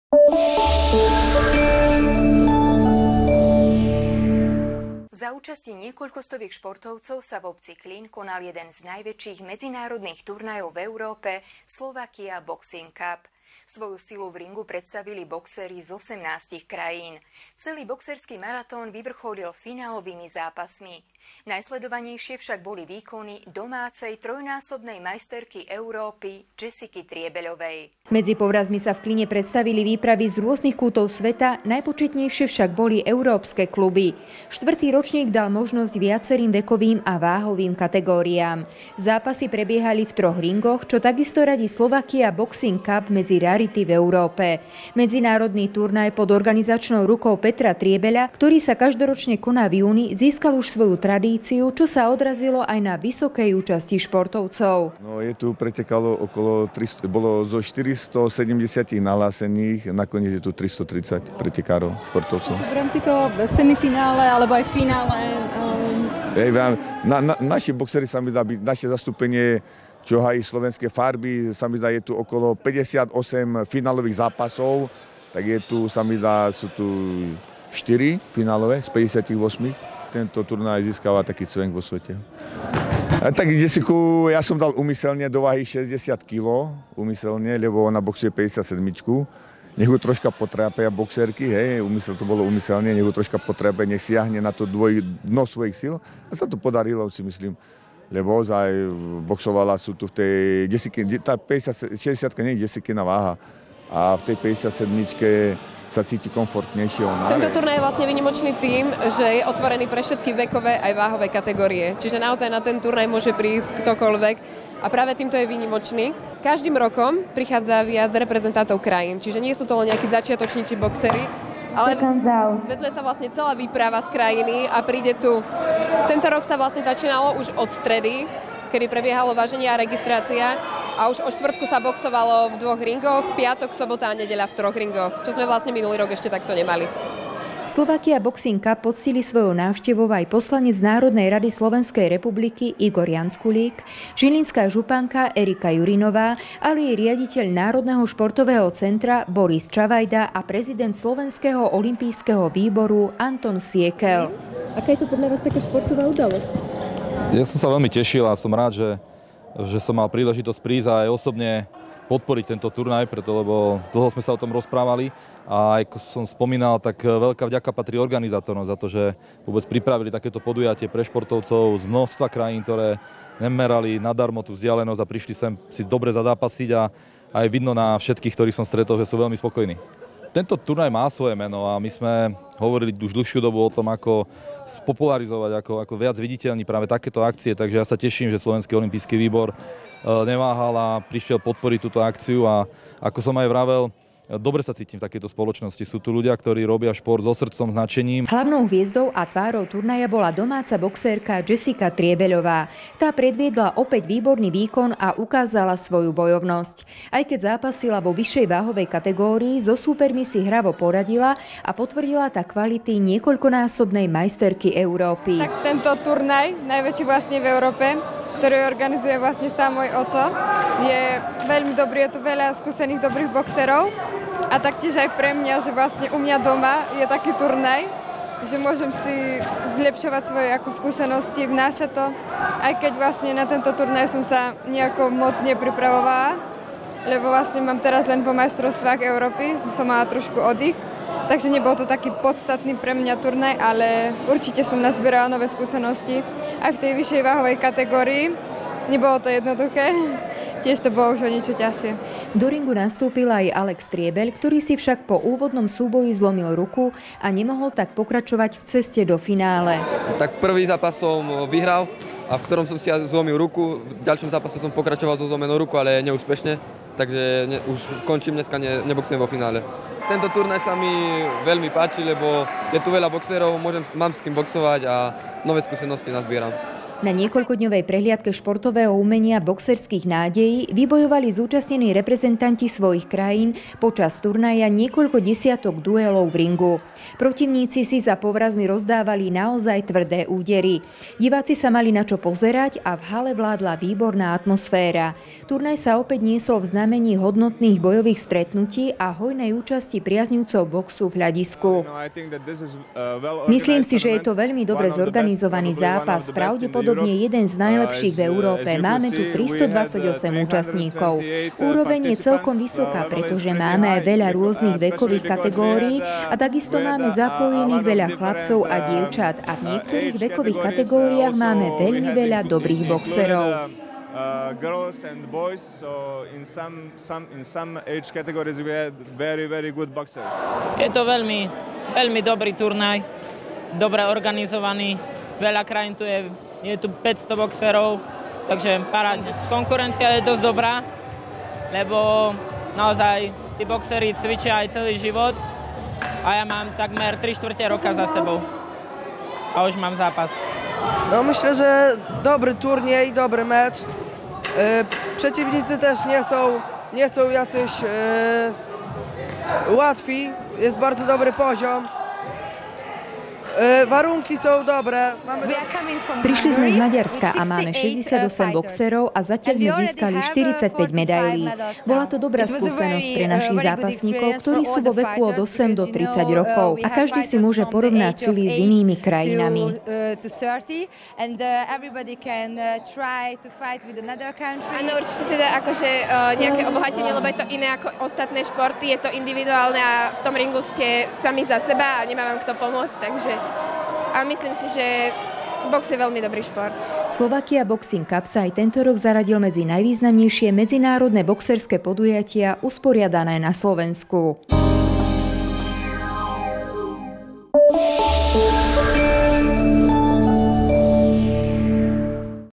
SLOVAKIA BOXING CUP
Za účasti 18 štátov sa v obci Klin konal jeden z najväčších medzinárodných turnajov v Európe SLOVAKIA BOXING CUP. Svoju silu v ringu predstavili boxeri z 18 krajín. Celý boxerský maratón vyvrcholil finálovými zápasmi. Najsledovanejšie však boli výkony domácej trojnásobnej Majsterky Európy Jessicy Triebeľovej.